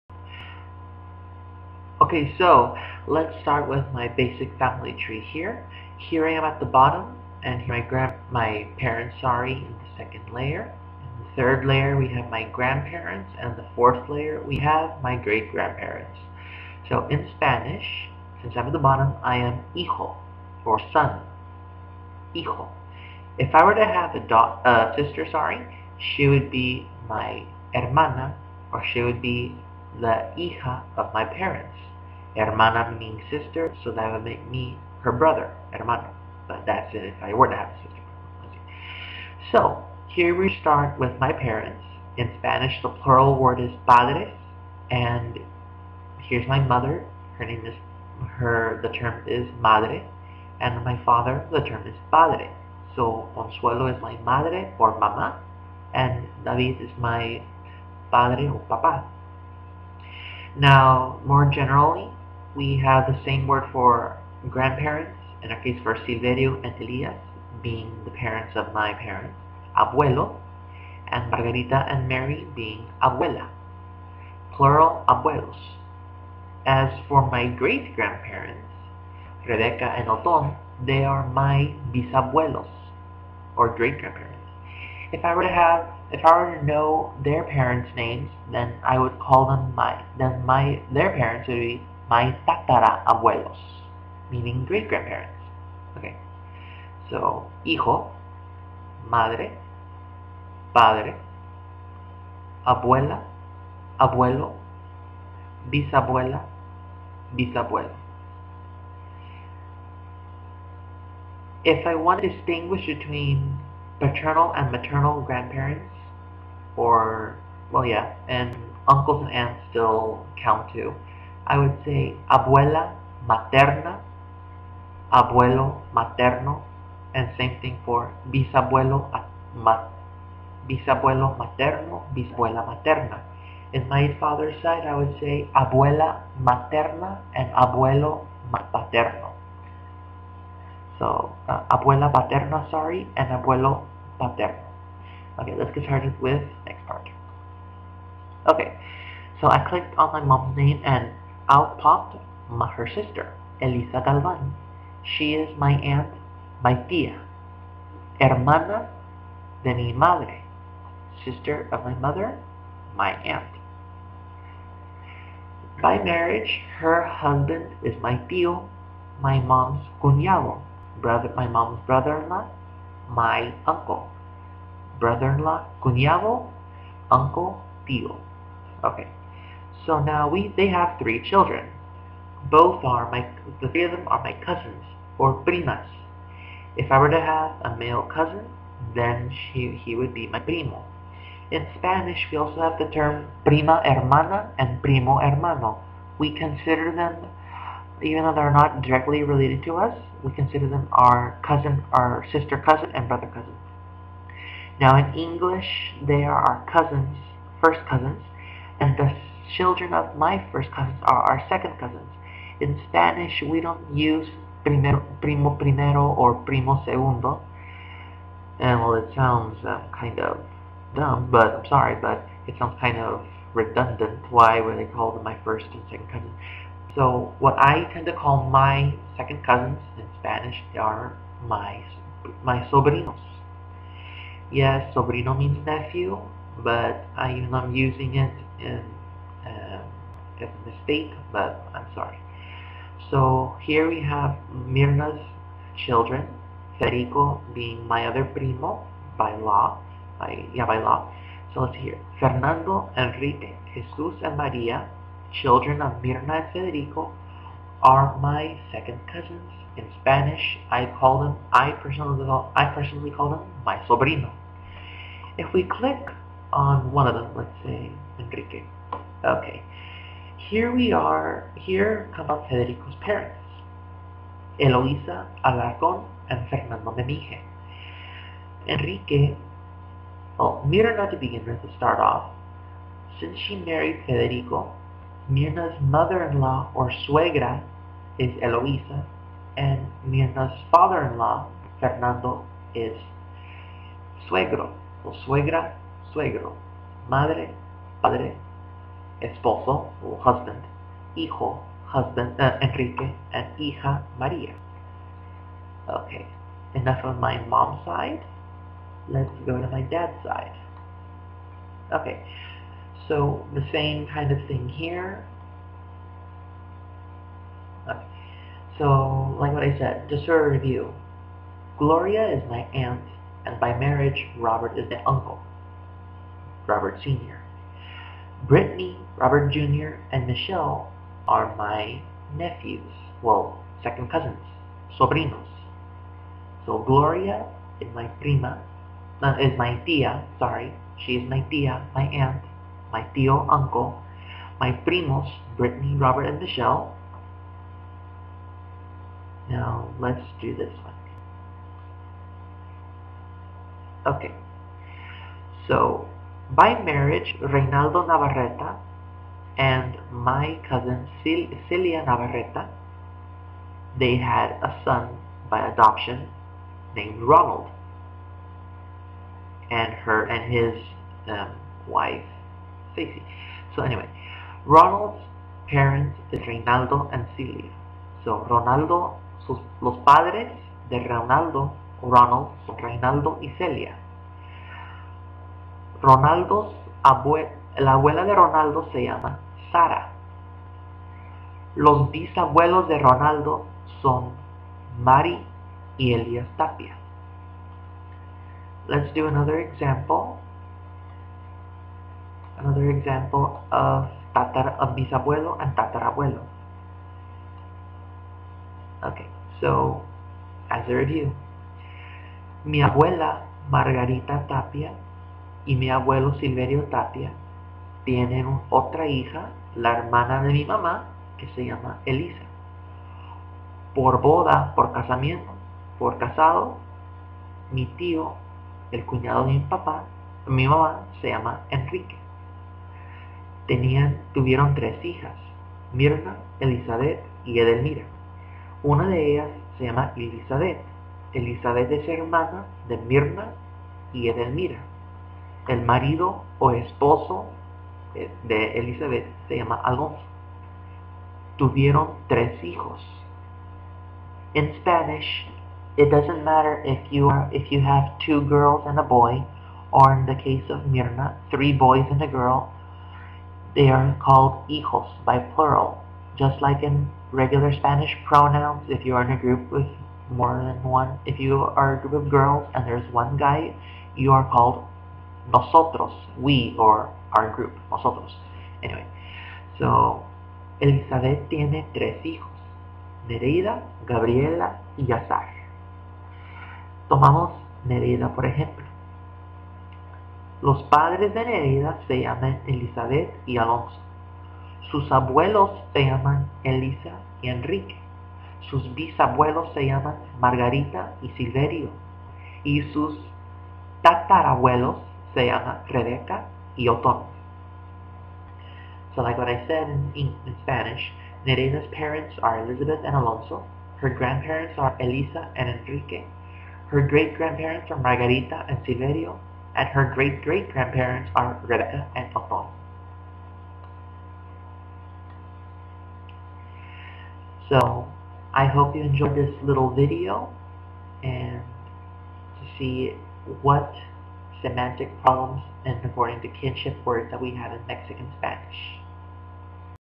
Kinship Terms in (Mexican) Spanish
Terminos de Parentesco

En orden / In order
Hijo - Son
Hija - Daughter
Hermana - Sister
Hermano - Brother
Padres - Parents
Madre- Mother
Padre- Father
Abuelo - Grandfather*
Abuela - Grandmother*
Bisabuelo - Great-Grandfather*
Bisabuela - Great Grandmother*
Tatarabuelo - Great Great Grandfather*
Tatarabuela - Great Great Grandmother*
Tia - Aunt*
Tio- Uncle*
Cunado - Brother in Law
Cunada -Sister in Law
Esposo / Marido -Husband
Primo / Primo-Hermano - Cousin
Sobrino - Nephew
Suegro - Father in Law
Suegra - Mother in Law

Not mentioned:
Madrastra - Stepmother
Padrastro - Stepfather
Hijastro - Stepson
Hijastra - Stepdaughter
Padrino - Godfather
Madrina - Godmother
Ahijado - Godson
Ahijada -Godaughter

* To distinguish between sides in the family, you can always say
"Materno/a" for Maternal and "Paterno/a" for Paternal
Example: Tio Materno (Maternal Uncle), and Abuela Paterna (Paternal Grandmother)